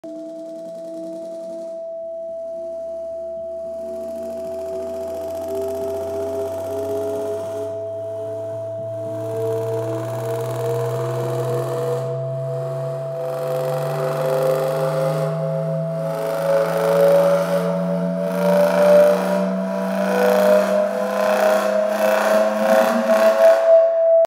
Black Hole Horizon
The 3D sound installation, Black Hole Horizon, takes us inside Extreme Light’s artificial black hole experiment. It transforms real data from the lab into a an 8-speaker sound environment. As we move through the space, we are surrounded by a flowing vortex of sound, which increases in pitch and velocity until the point where we meet the event horizon. Created from original data sourced from Extreme Light's analogue black hole experiment at Heriot-Watt University UK. Exhibited in 'Data Horizons' for the Edinburgh International Science Festival in 2017. Video production by Alex Ingle 2017.